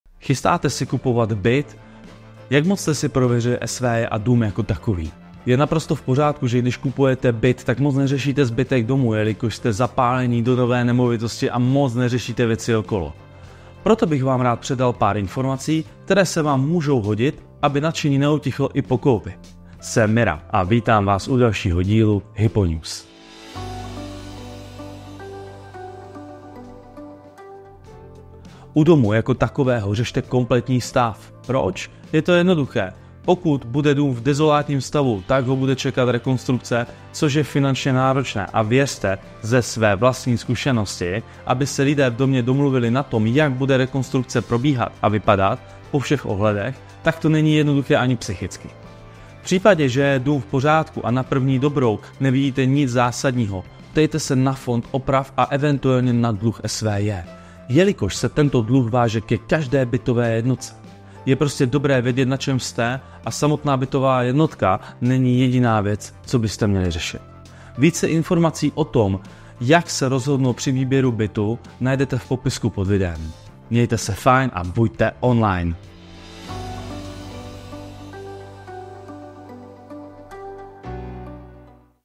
HypoNews: Nenaleťte realitním makléřům, kontrolujte stav SVJ a domu jako celku
Plánujete koupit byt? Jak pečlivě jste prověřili společenství vlastníků jednotek (SVJ) a stav domu? Je pochopitelné, že se při koupi bytu zaměřujete hlavně na samotnou nemovitost a neřešíte tolik ostatní aspekty. Rád bych vám poskytl pár rad, které vám mohou pomoci, aby vaše nadšení vydrželo i po koupi.

Stav domu je důležitý. Proč? Pokud je dům v špatném stavu, čeká ho rekonstrukce, což je finančně náročné a domluva mezi sousedy o jejím průběhu může být psychicky náročná.

Pokud se dům zdá v pořádku, zjistěte si informace o fondu oprav a případných dluzích SVJ, protože tyto dluhy se týkají každé bytové jednotky. Je dobré mít přehled o všem, co s koupí souvisí, nejen o samotném bytu.

Více informací o tom,  jak se rozhodnout při výběru bytu najdete na